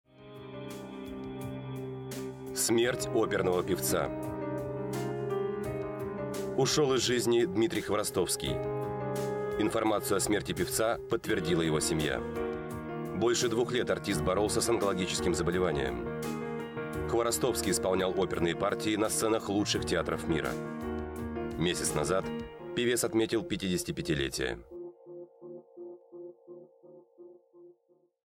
Скончался оперный певец Дмитрий Хворостовский
Ушёл из жизни Дмитрий Хворостовский